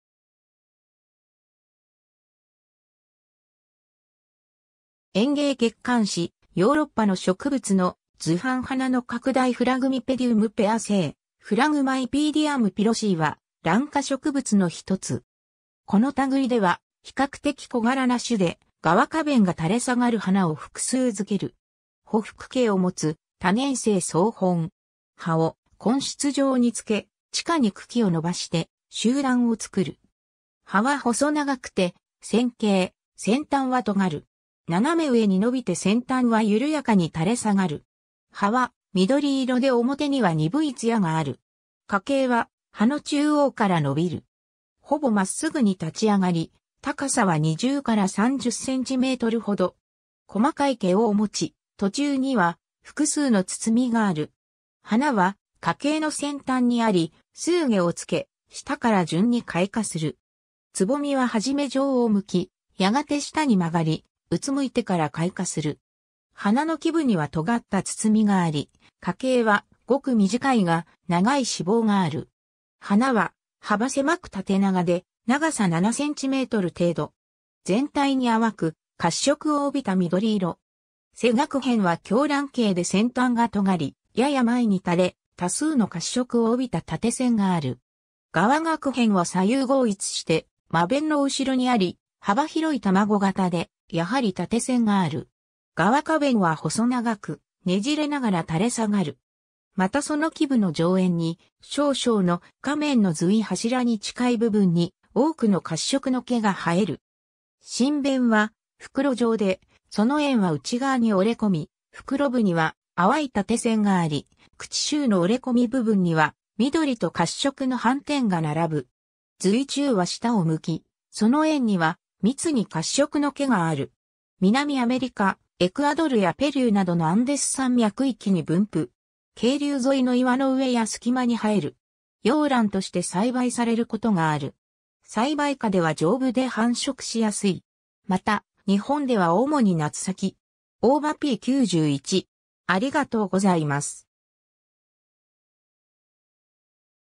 フラグミペディウム・ペアセイ
園芸月刊誌『ヨーロッパの植物』の図版 花の拡大 フラグミペディウム・ペアセイ Phragmipedium pearcei は、ラン科植物の一つ。
この類では比較的小柄な種で、側花弁が垂れ下がる花を複数着ける。
 匍匐茎を持つ多年生草本。
葉を根出状につけ、地下に茎を伸ばして集団を作る。
 葉は細長くて線形、先端は尖る。
斜め上に伸びて先端は緩やかに垂れ下がる。
葉は緑色で表には鈍いつやがある。
 花茎は葉の中央から伸びる。
ほぼ真っ直ぐに立ち上がり、高さは20-30cmほど。
細かい毛を持ち、途中には複数の包がある。
花は花茎の先端にあり、数花をつけ、下から順に開花する。
蕾は始め上を向き、やがて下に曲がり、俯いてから開花する。
鼻の基部には尖った包があり、花茎はごく短いが、長い子房がある。
 花は幅狭く縦長で、長さ7cm程度。
全体に淡く褐色を帯びた緑色。
背萼片は狭卵形で先端が尖り、やや前に垂れ、多数の褐色を帯びた縦線がある。
側萼片は左右合一して真弁の後ろにあり、幅広い卵形で、やはり縦線がある。
側花弁は細長く、ねじれながら垂れ下がる。
またその基部の上縁に少々の、下面のずい柱に近い部分に多くの褐色の毛が生える。
唇弁は袋状で、その縁は内側に折れ込み、袋部には淡い縦線があり、口周の折れ込み部分には緑と褐色の斑点が並ぶ。
ずい柱は下を向き、その縁には密に褐色の毛がある。
 南アメリカ、エクアドルやペルーなどのアンデス山脈域に分布。
渓流沿いの岩の上や隙間に生える。
 洋ランとして栽培されることがある。
栽培下では丈夫で繁殖しやすい。
また日本では主に夏咲き。
 ^ 大場(2010)p.91